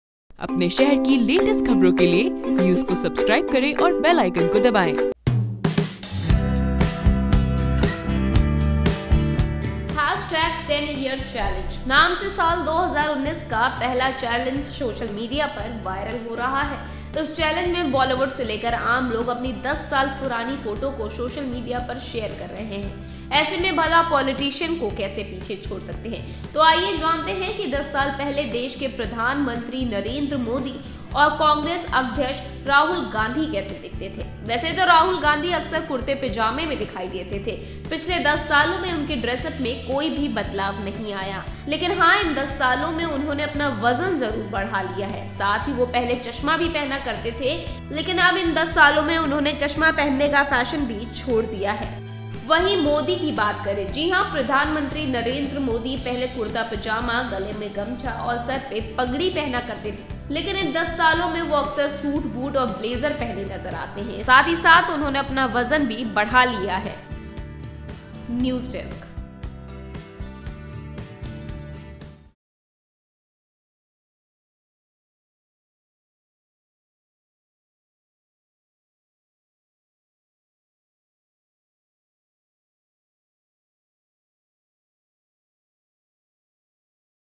10YearChallenge: 10 साल पहले ऐसे दिखते थे PM Modi और Rahul Gandhi
10YearChallenge नाम से साल 2019 का पहला चैलेंज सोशल मीडिया पर वायरल हो रहा है। इस चैलेंज में बॉलीवुड से लेकर आम लोग अपनी 10 साल पुरानी फोटो सोशल मीडिया पर शेयर कर रहे हैं ऐसे में हम भला पॉलिटिशंस को कैसे पीछे छोड़ सकते है आइये जानते है कि 10 साल पहले देश के प्रधानमंत्री नरेंद्र मोदी और कांग्रेस अध्यक्ष राहुल गांधी कैसे दिखते थे।